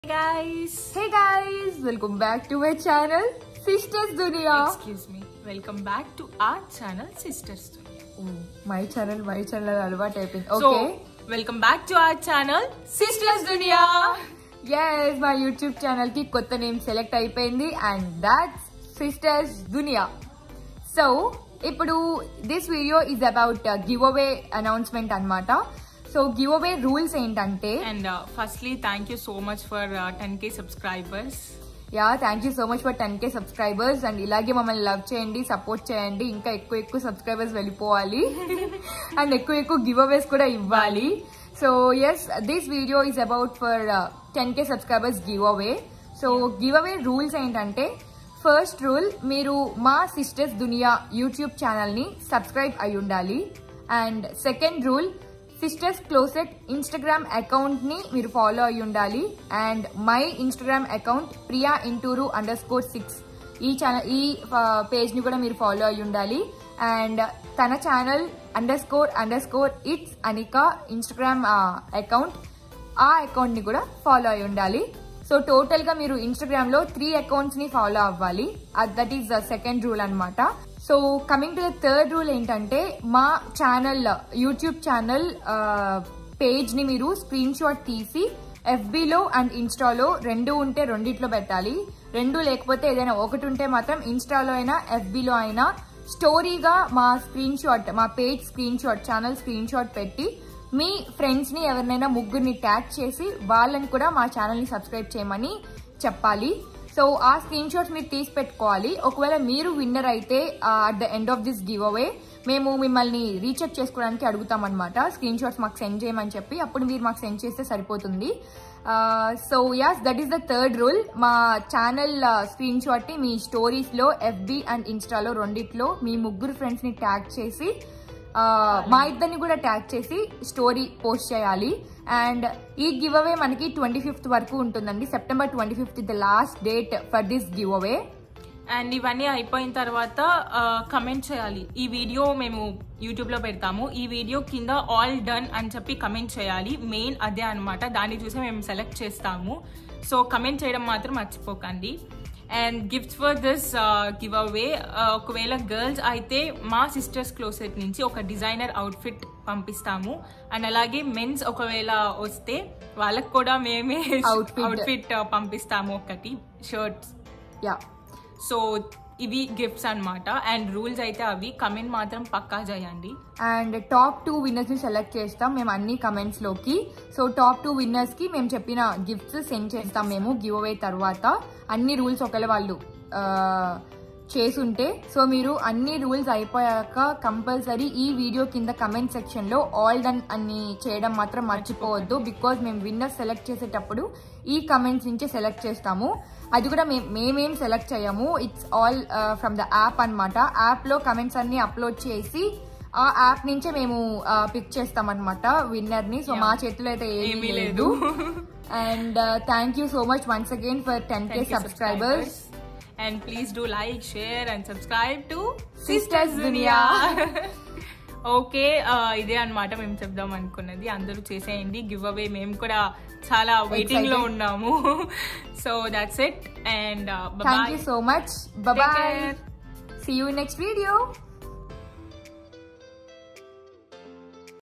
2nd Giveaway || Sisters Dhuniya || priya inturu || Its anika
Hello lovlies. so here is our second giveaway announcement for our 10k subscribers!! Yay!! Thankyou so much for your love and support❤️
2. You should follow priyainturu_6 , __itsanika , sisterscloset_1 Instagram accounts.
4. After completing all the rules comment ALL DONE in the comment section of this video .
Top 2 winners will get beautiful designer outfits from our sisterscloset boutique.
Links to follow:
Priyainturu insta :